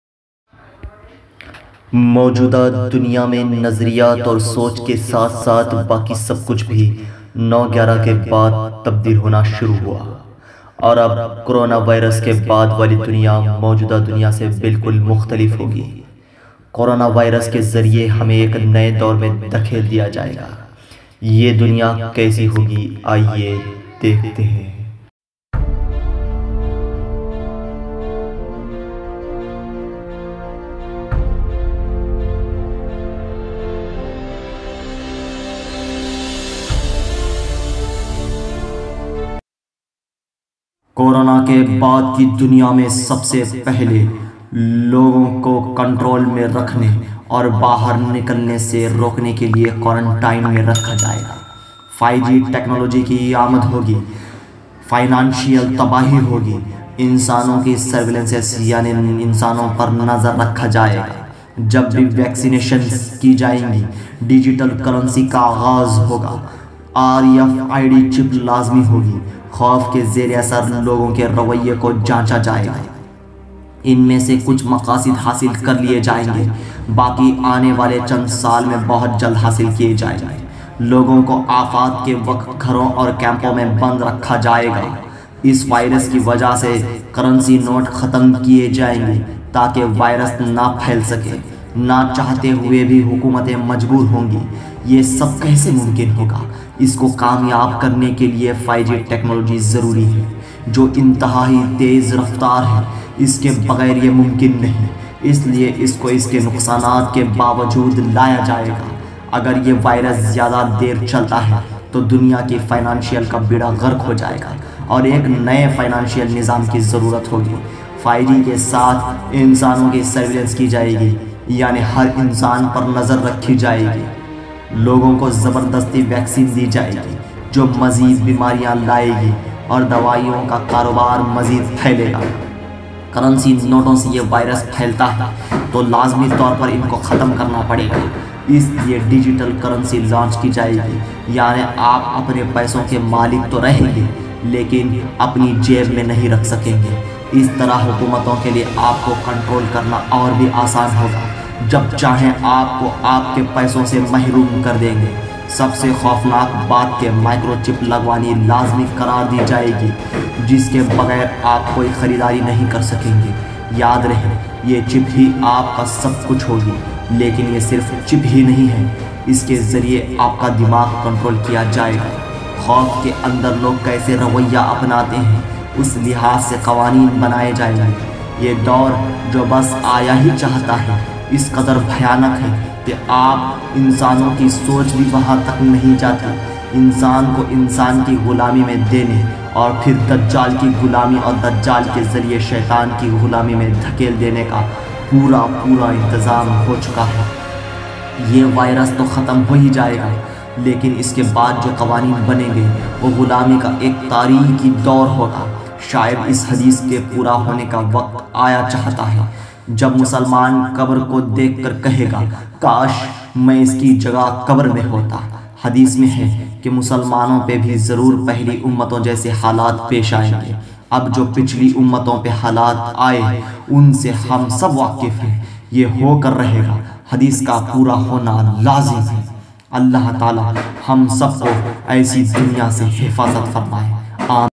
The world after corona | corona ke baad ki duniya | MBI
Is videi me coronavirus khatam hone ke baad wali duniya ke baare me bataya gaya hai jab aap dekheinge to aap ke hosh ud jayeinge . Agar video achi lage to like karein aur apke sab dosto ko share karein.